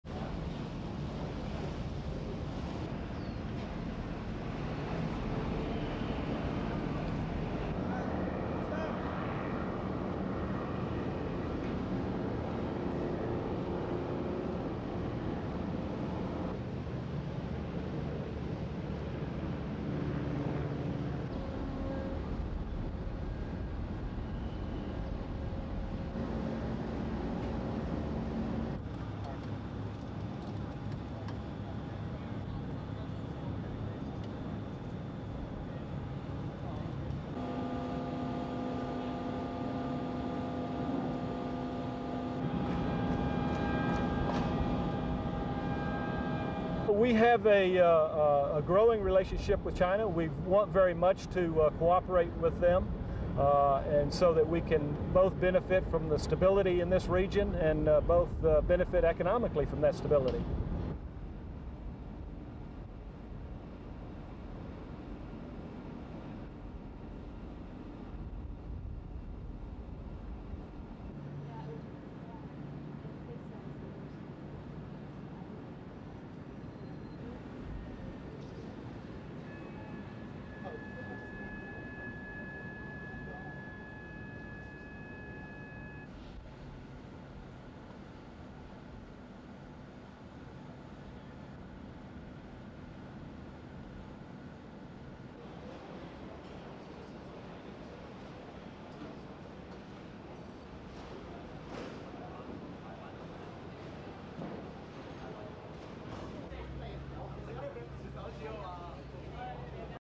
HONG KONG: SEVENTH FLEET FLAGSHIP USS BLUE RIDGE ARRIVES ON VISIT
The flagship of the U-S Seventh Fleet arrived in Hong Kong Monday, the first foreign navy to call on the former British colony since it was returned to China last month.

The Blue Ridge docked in Victoria Harbour on the Kowloon side early in the day.

Officials said the call was a purely social one - to give the Japan-based crew some rest and relaxation.

U-S-S Blue Ridge - the command and control ship of the U-S Seventh Fleet - arrived in Hong Kong Monday morning, one day after Typhoon Victor swept through the territory.

This has been the first foreign naval vessel visiting the territory since change of sovereignty on July 1.

With around 900 crewmen on board, the 620 feet (189 metres) long ship weighs 19,200 tons at full capacity.

The Blue Ridge operates as a command ship for amphibious task force and landing force commanders during all phases of fleet-wide operations.

Even after Hong Kong's return to China, U-S Commander Robert Natter says he sees no change in their role over the territory.

SOUNDBITE (English):
"We have a growing relationship with China.  We want very much to cooperate with them and so that we can both benefit from the stability in this region and both benefit economically from that stability."
SUPER CAPTION: Robert J. Natter, US Navy Vice Admiral, Commander, United States Seventh Fleet

The commander is scheduled to pay the People's Liberation Army (PLA) a courtesy call on Tuesday.

He expressed interest in engaging the P-L-A troops in some sports and social activities during the visit.

The fleet's area includes 52 (m) million square miles of the Pacific and Indian Oceans - stretching from the mid-Pacific to the east of African coast, and from the Kurile Islands in the north to the Antarctic in the South.

The Blue Ridge - which is based in Yokosuka, Japan - makes frequent port calls throughout the western Pacific including Hong Kong, Singapore, Thailand and Australia.
It is due to stay in Hong Kong for a week, where the crew will take in some rest and relaxation.